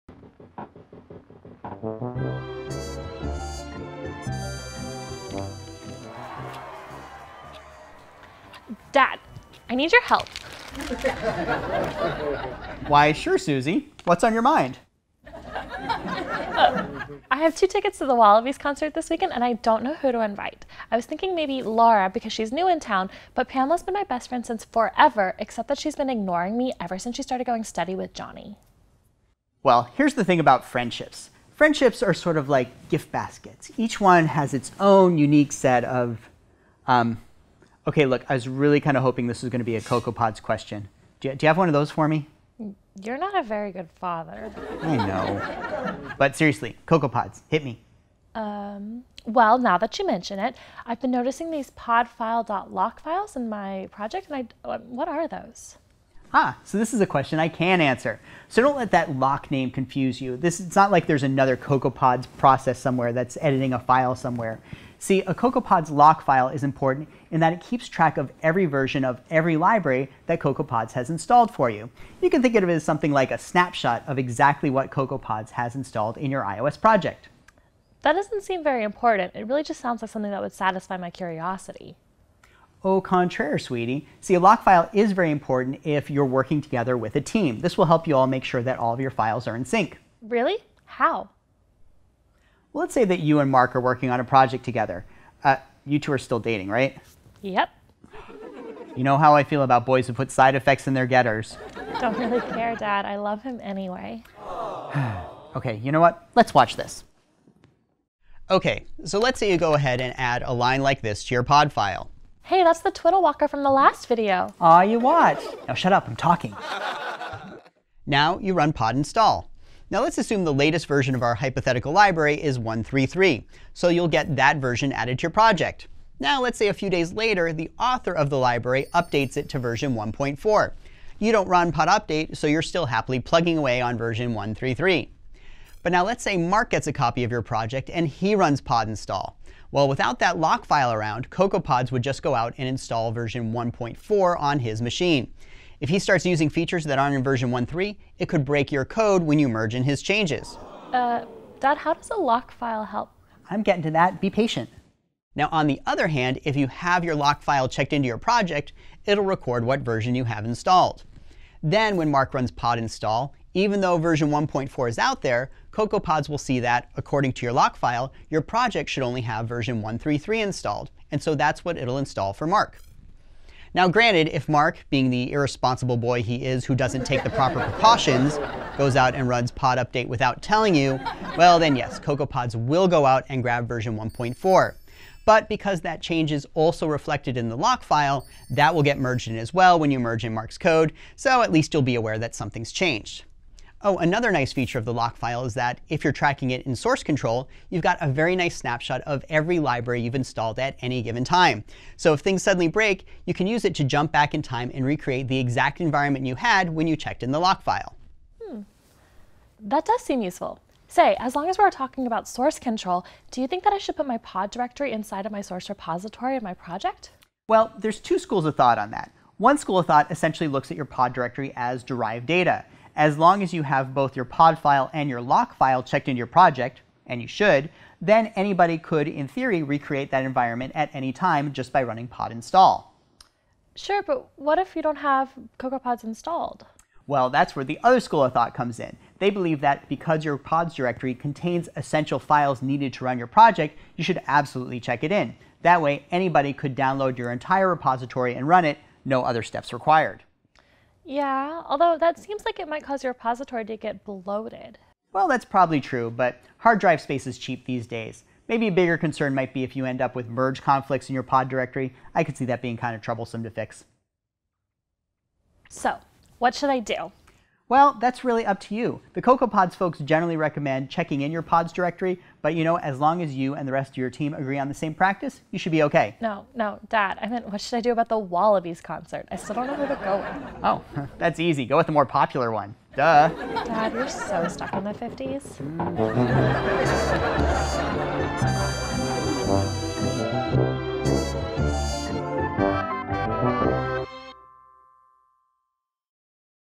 CocoaPods and Lockfiles (Route 85)
In this classic episode of Route 85, Todd explains to Suzie what a lockfile is for when you start using CocoaPods in your iOS project, and whether or not you should include your pods directory in source control.